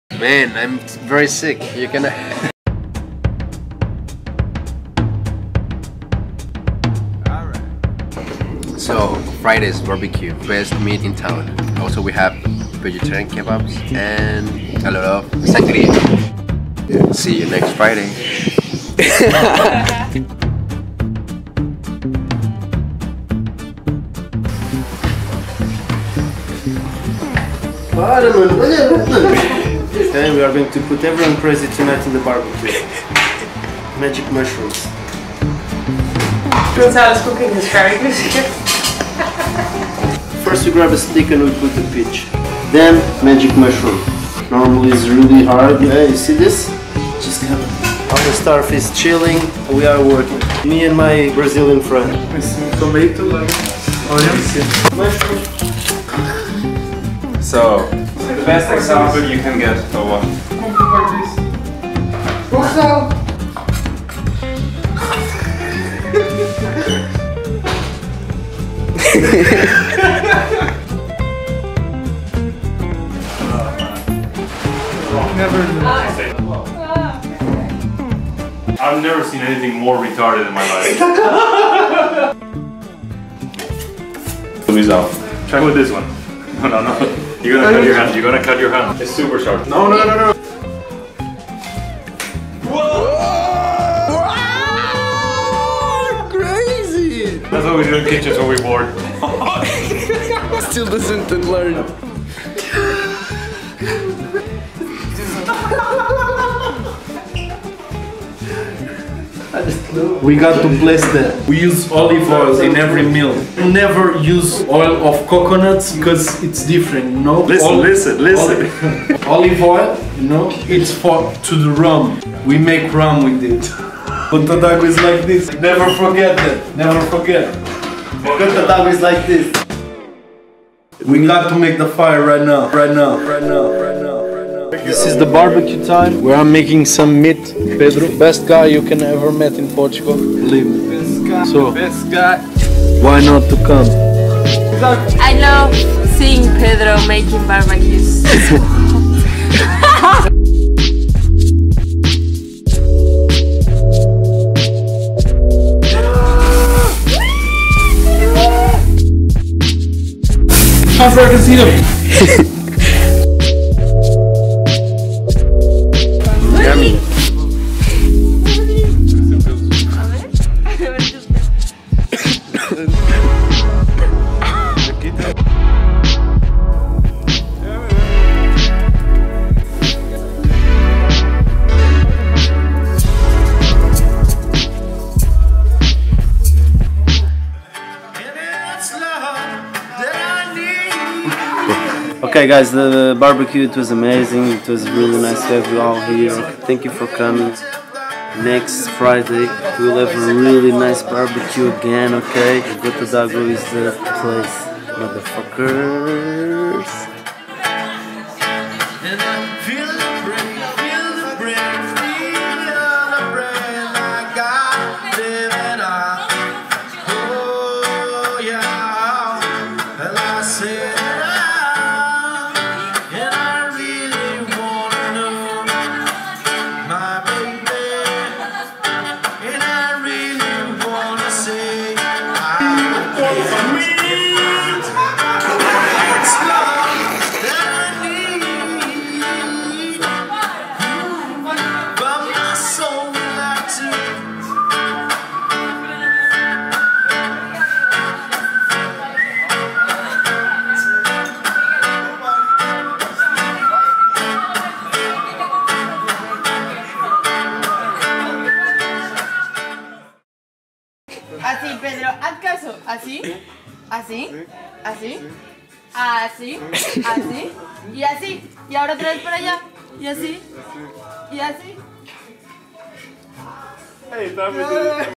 Best bbq in the world | Gota Dagua Surf Camp
Check this behind de scenes of one of our Incredibles BBQ in Gota Dagua Surf Camp, where we always have good food and music for our surf family! Yeeeeeew!

SurfCamp - SurfSchool - Yoga